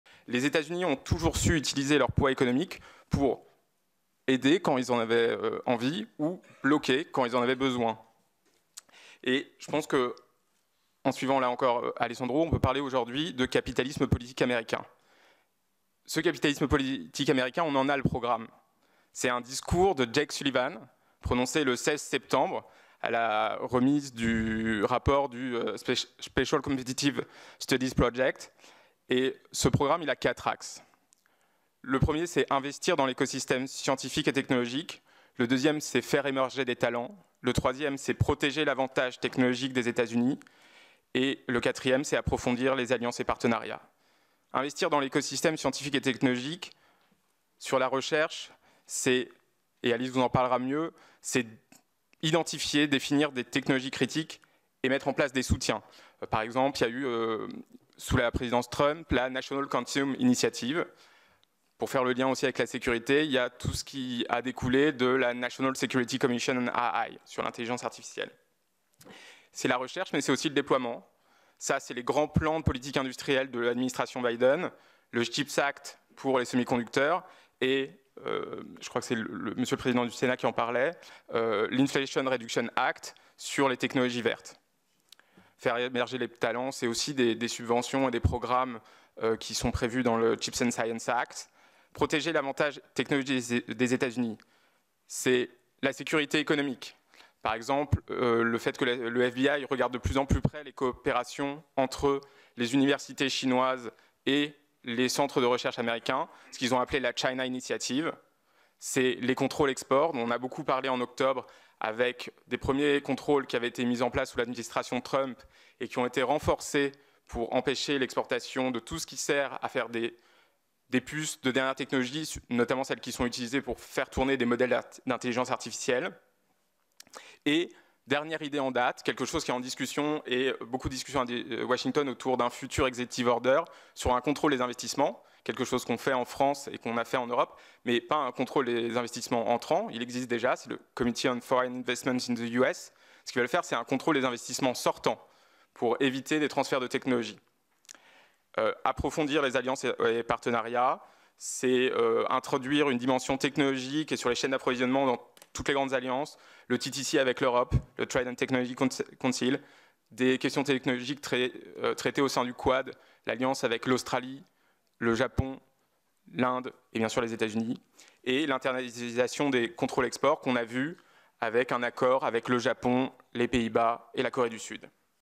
Les 4 axes du capitalisme politique américain
Mercredi 15 février 2023, le Sénat a organisé les Rencontres du Grand Continent, regards croisés sur les enjeux du monde contemporain sur le thème "Rivalités Chine-États-Unis : la France et l'Europe face aux recompositions géopolitiques", autour de :
- Gilles Kepel, politologue, directeur de la Chaire Méditerranée Moyen-Orient à l’École normale supérieure-PSL ;
- Louis de Catheu, co-directeur scientifique de la série "Capitalismes politiques en guerre" pour Le Grand Continent, spécialiste des politiques publiques dans le domaine du numérique et des nouvelles technologies ;
- Alice Pannier, responsable du programme Géopolitique des technologies (Ifri) ;
- Sébastien Jean, professeur d’économie, titulaire de la Chaire d’économie industrielle Jean-Baptiste Say au CNAM.

Louis de Catheu a développé les 4 axes de la stratégie du capitalisme politique américain.